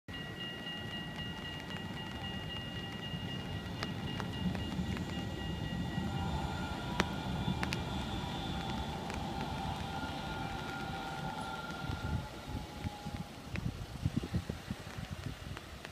Tram level crossing near Rotterdam Central Station
A typical road/tram level crossing in Rotterdam with flashing lights and warning bells. At every intersection that I saw, trams had absolute priority over all other modes.